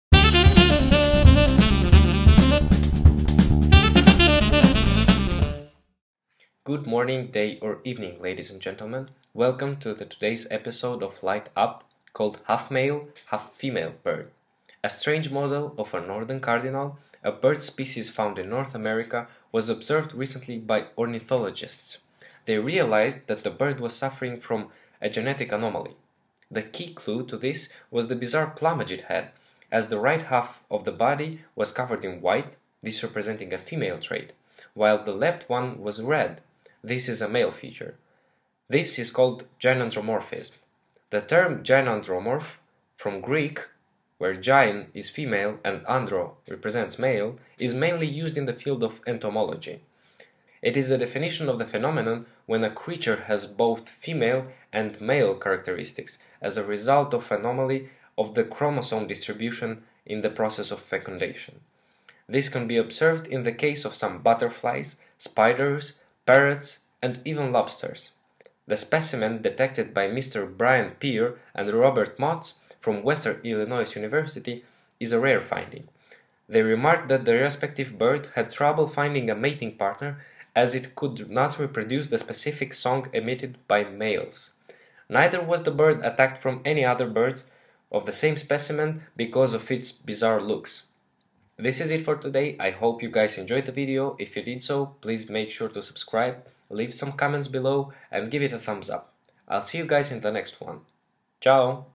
Half male half female Northern Cardinal
A strange model of a northern Cardinal, a bird species found in North America, was observed recently by ornithologists. They realized that the bird was suffering from a genetic anomaly. The key-clue was the bizarre plumage as the right half of the body was white (this representing a female trait) while the left one was red (this is a male feature)! This is called Gynandromorphism

The term gynandromorph, from Greek "gyne" female and "andro" male, is mainly used in the field of entomology.
It is the definition for the phenomenon when a creature has both male and female characteristics, as a result of an anomaly of the chromosome distribution after fecundation.

This can be observed in the case of some butterflies, parrots and even lobsters.

The specimen detected by Brian Peer and Robert Motz from Western Illinois University is a rare finding. They remarked that the respective bird had trouble finding a mating partner as it could not reproduce the specific song emitted by males. Neither was it attacked by birds of the same speciment because of the unusual aspect it had.